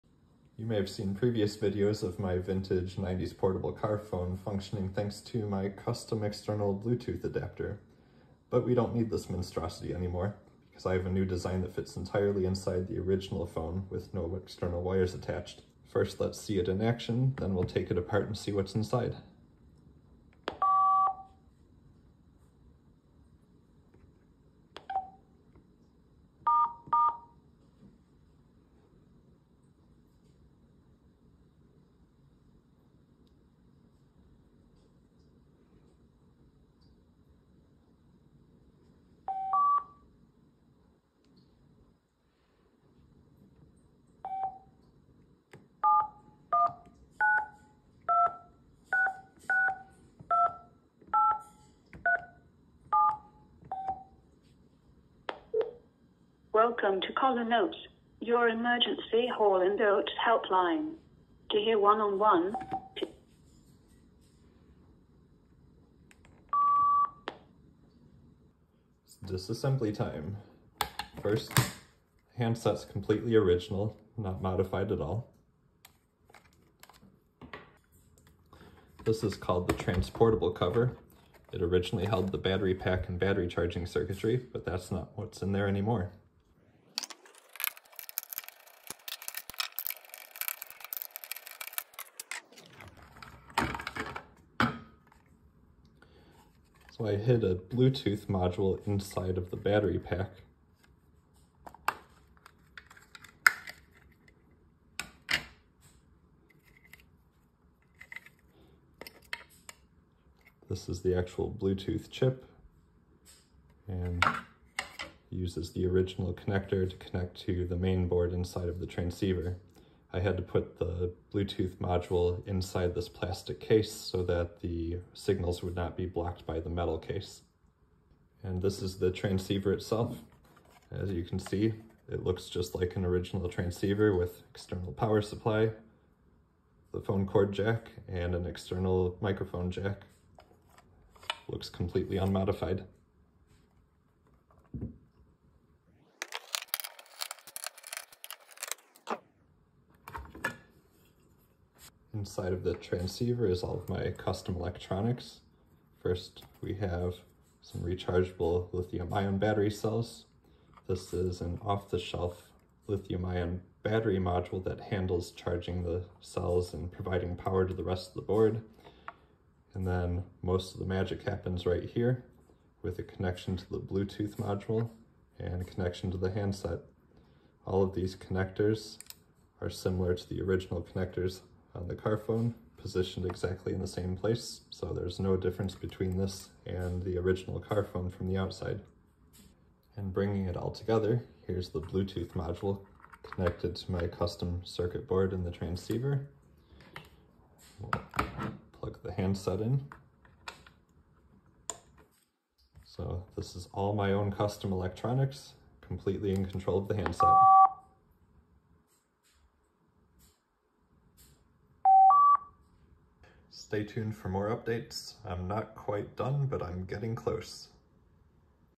New vintage car phone Bluetooth adapter demo/disassembly
This is my new custom Bluetooth adapter design for my vintage portable car phone. This new design is contained entirely inside of the original car phone’s transceiver so that it appears completely original from the outside.

Apologies for the vertical video. I recorded this for Instagram and TikTok and am too lazy to film the whole thing again in landscape mode.

Chapters:
00:00 - Intro
00:15 - Proof that it works
01:08 - Disassembly

The phone is able to make/receive real calls thanks to my custom Bluetooth adapter. Full details of my project are documented